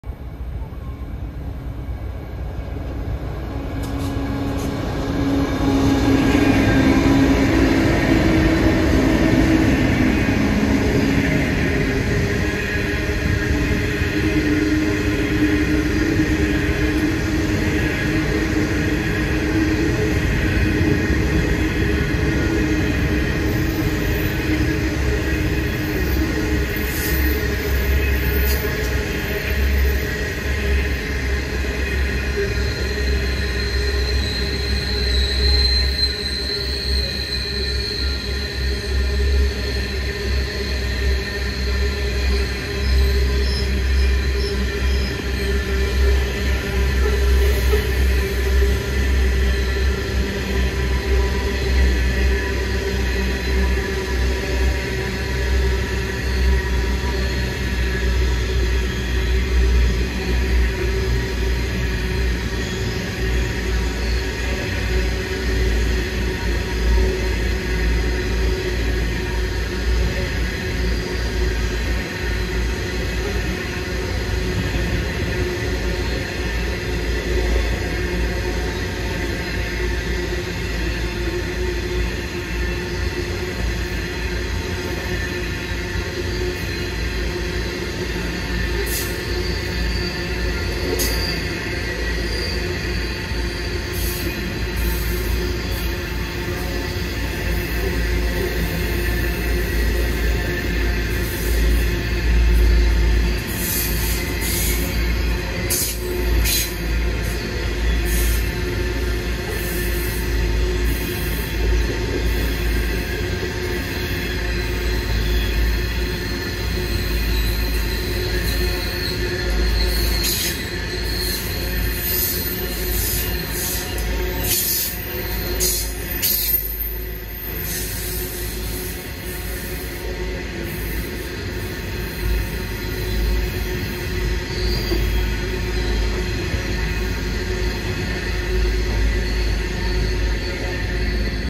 Pacific National 9028, LDP003 & 9027 at Telarah - 6/6/24
Pacific National’s 9028, LDP003 & 9027 pass through Telarah with loaded coal MT372 at 9:08am on Thursday 6th June, 2024.